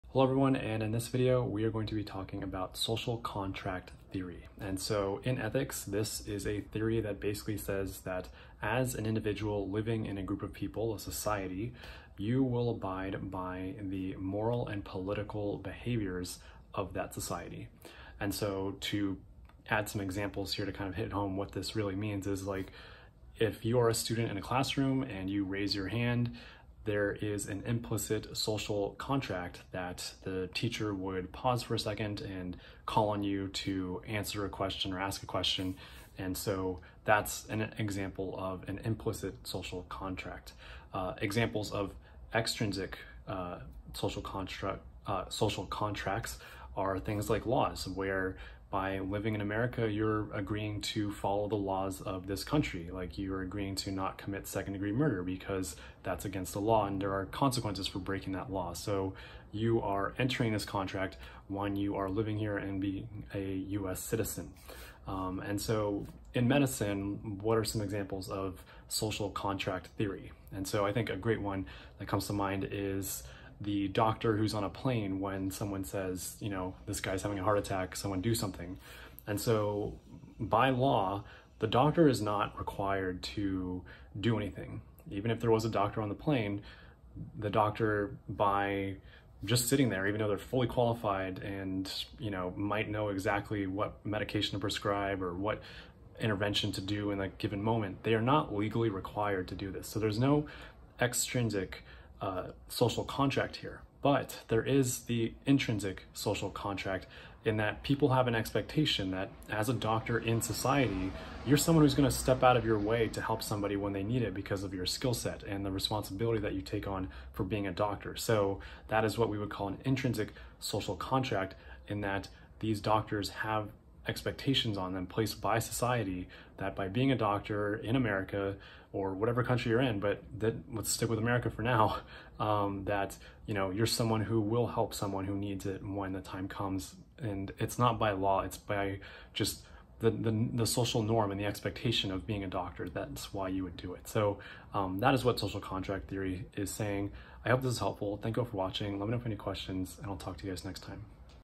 Social Contract Theory in Medical Ethics
I talk about Social Contract Theory and how it applies to Medical Ethics. I also describe the different between implicit and explicit social contracts.